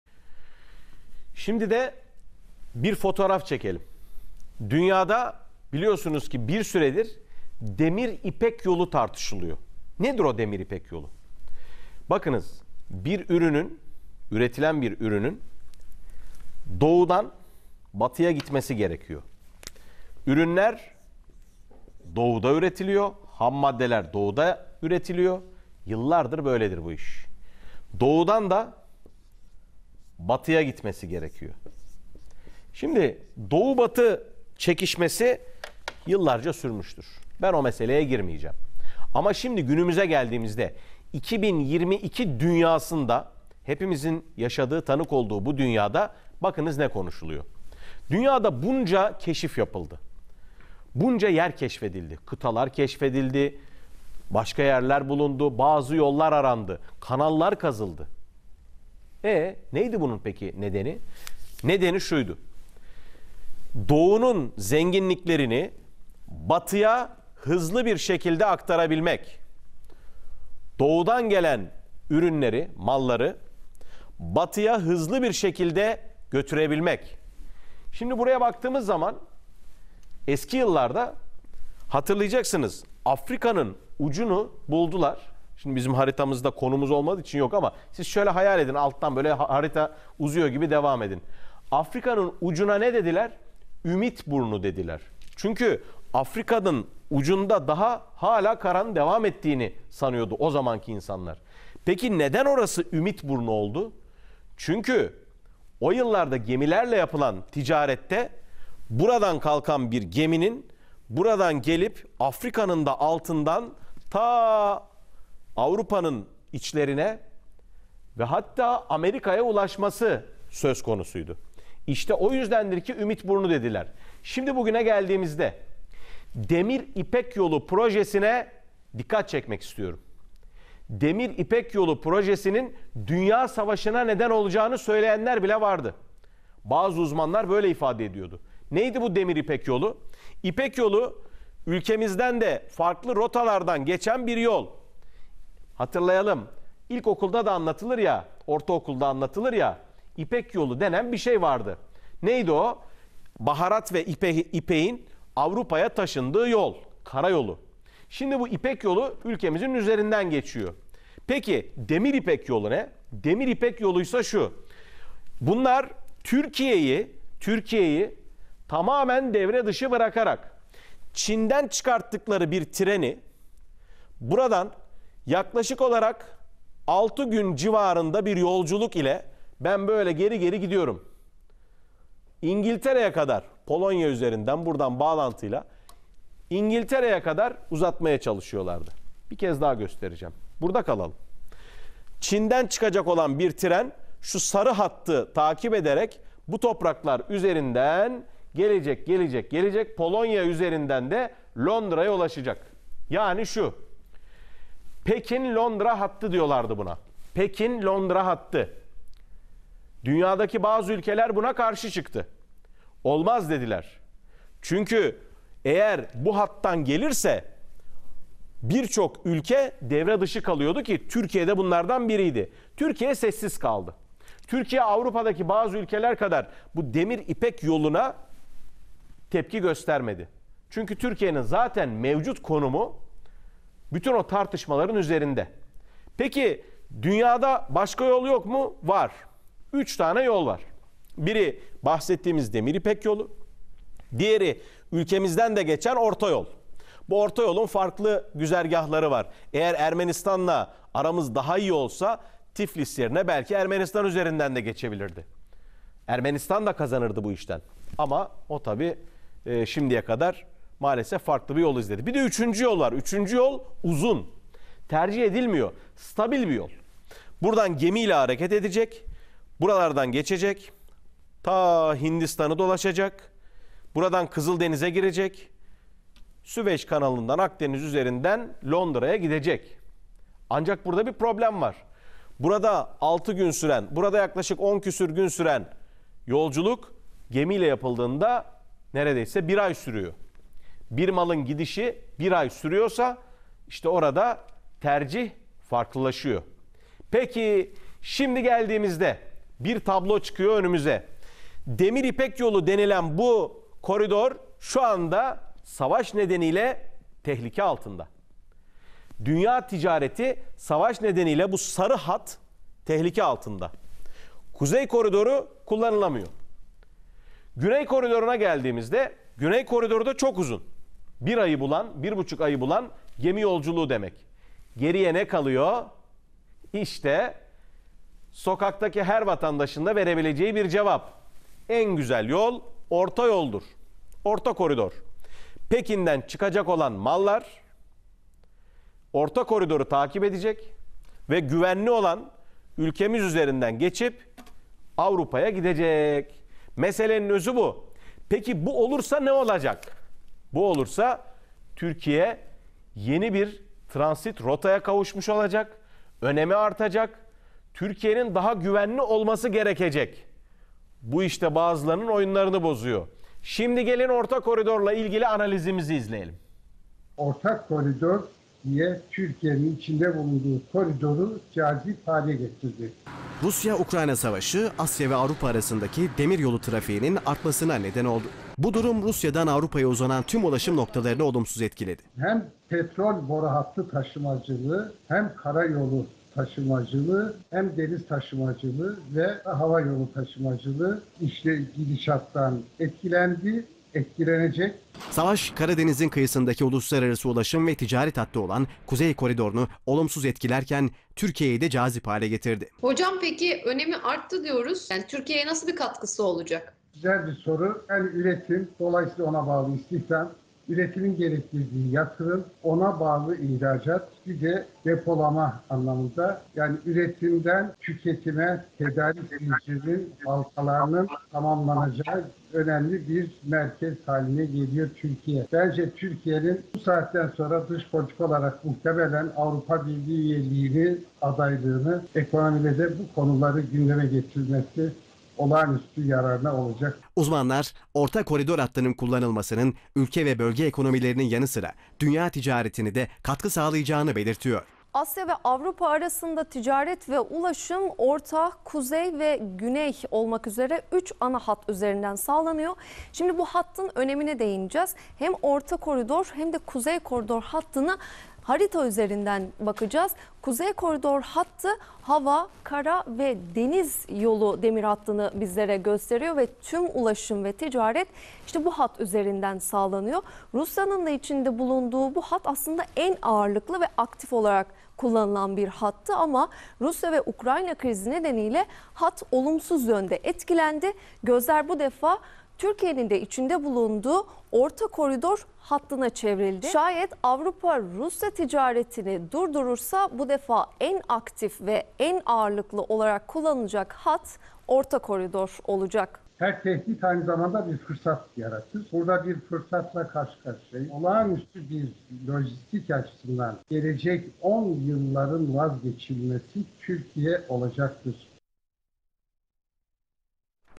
Türkiye'nin Jeopolitik Önemi Arttı! Uzman İsimler Anlattı
Ukrayna-Rusya gerilimi sonrası Doğu-Batı arasındaki güvenli ulaşım yolu için Türkiye'nin jeopolitik konumu daha da önem kazandı. Uzman isimler son gelişmelerin ülkemizi nasıl etkileyeceğini anlattı.